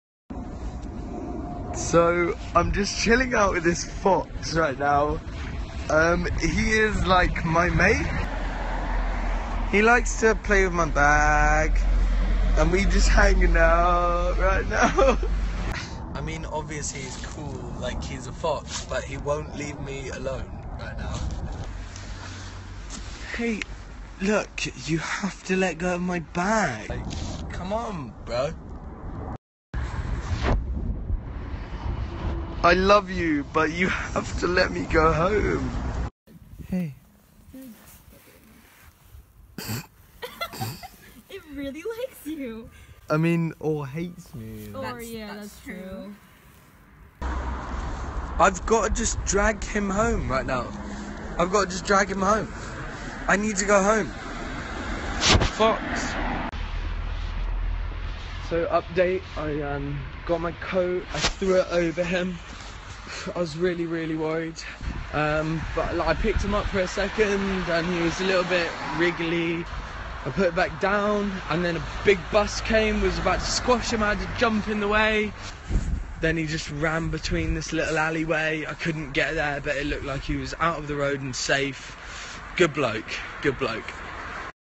Fantastic Fox and Friend Walk Through Streets of London
Henry Hoffman was trying to get home through the Hampton Wick area of London when he made a new pal. His fox friend accompanied him through the streets, becoming very attached to him. Eventually the fox went on his merry way, and Henry got home.

Video credit: YouTube/henry hoffman